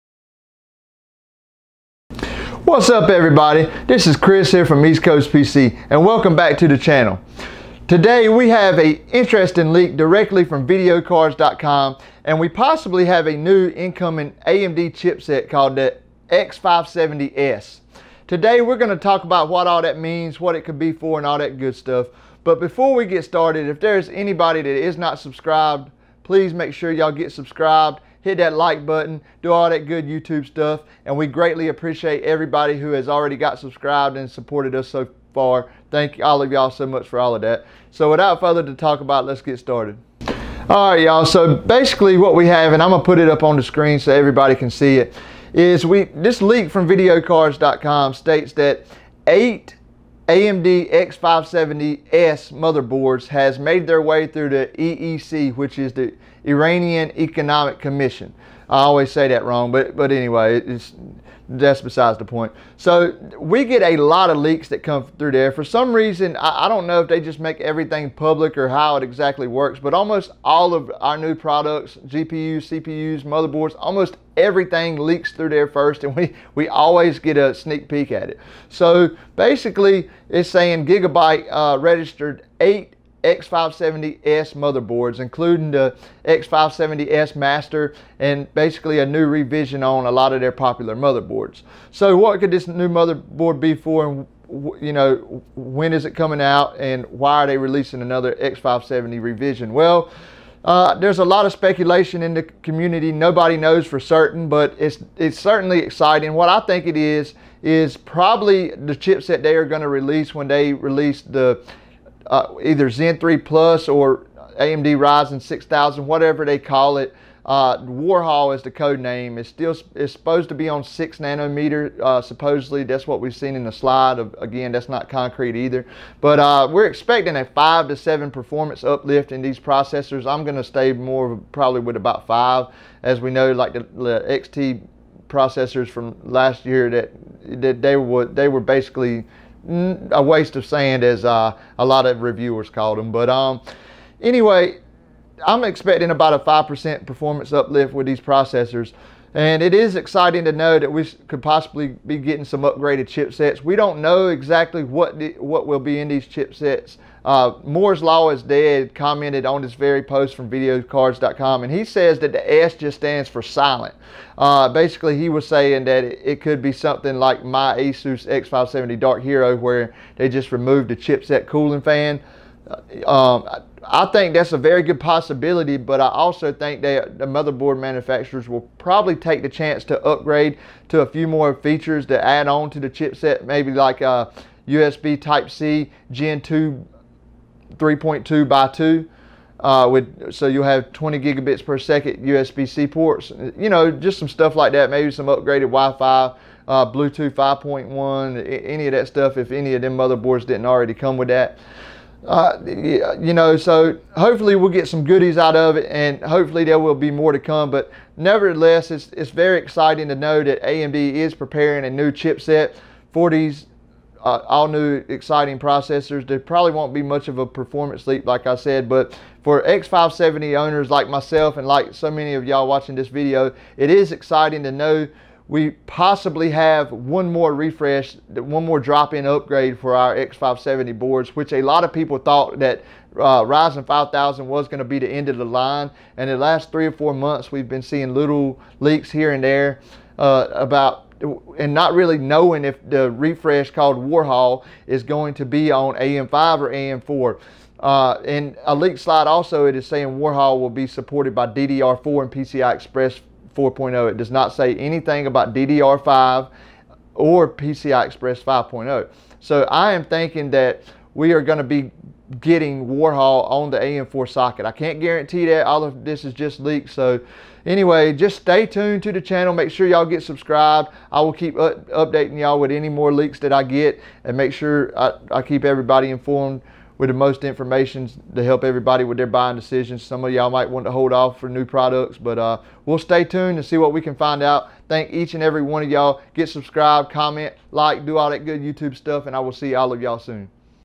AMD X570S Motherboards Leaked!!!
In This Video We Talk About A Leak From Videocardz.com About The Upcoming AMD X570S Chipset, Warhol Incoming??? Lets Talk About It!!!